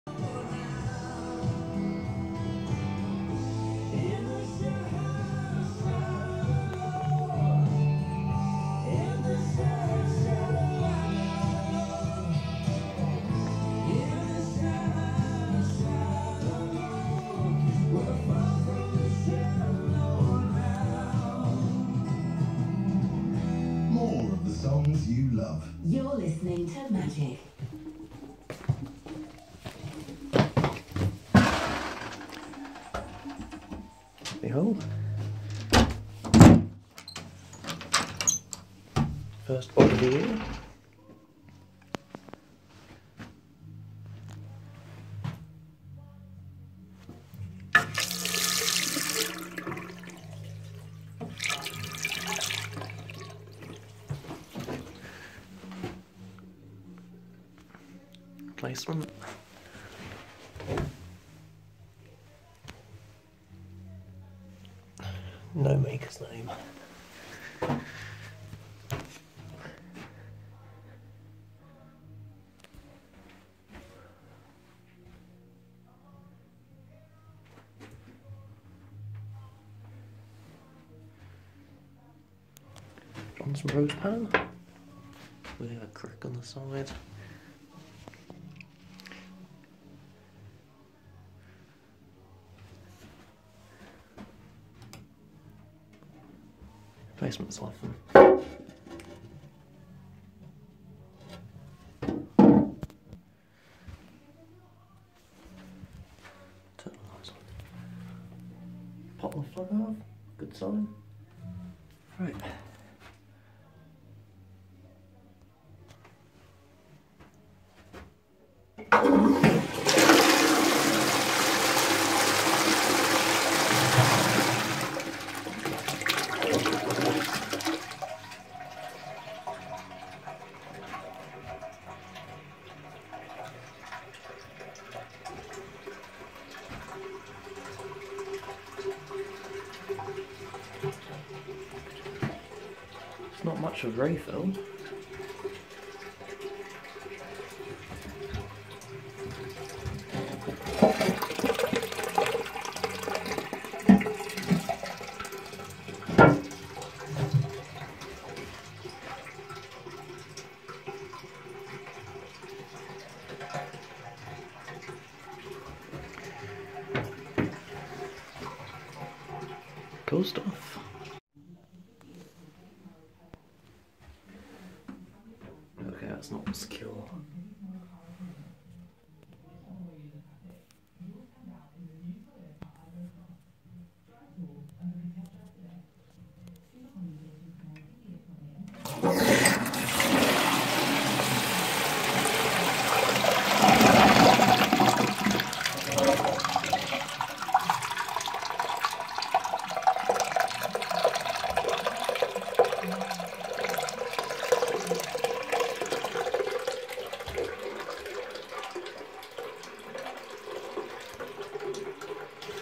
Early 1950s HLSP Shires Duranite & 1970s Johnson Bros Trentline pan
First film of the year, a permission visit round a local hall close to home.

First of all, what an amazing old place this is. Second of all, here's the first documentation of the year, enjoy. :D